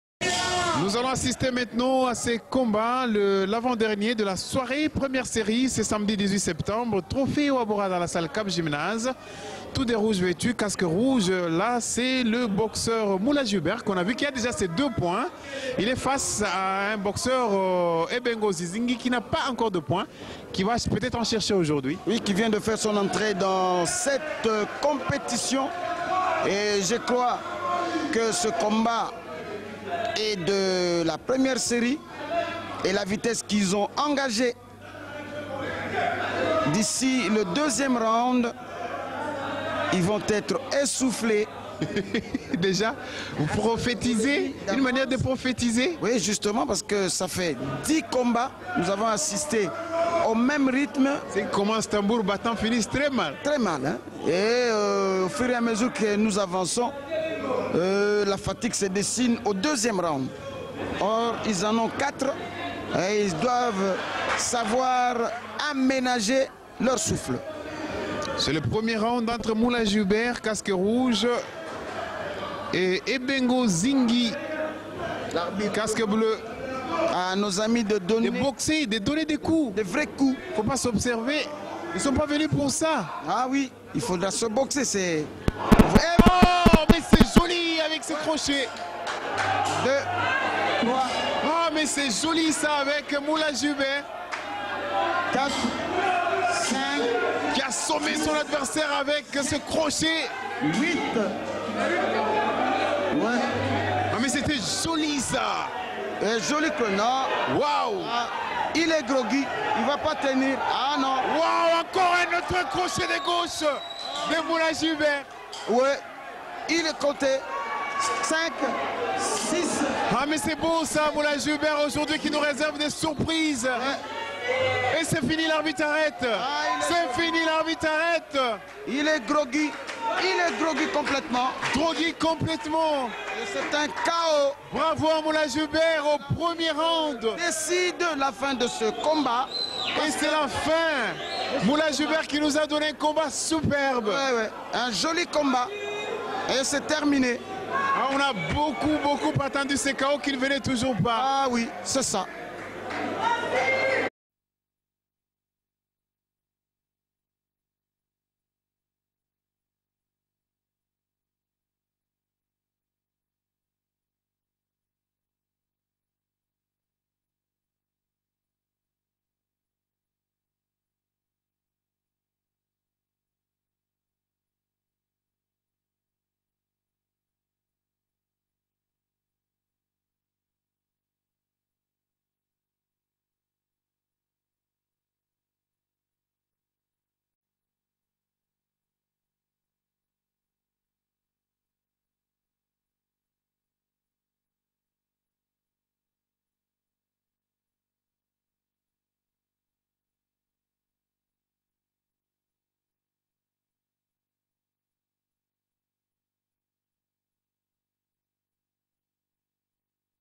2004 Trophée HBA - Hewa Bora Airways. Mulaji contre Ebengo
27 novembre 2004. Qualifications. Dans la salle Kab Gymnase au Stade des Martyrs de Kinshasa. 3 mois de boxe, 9 soirées le samedi soir.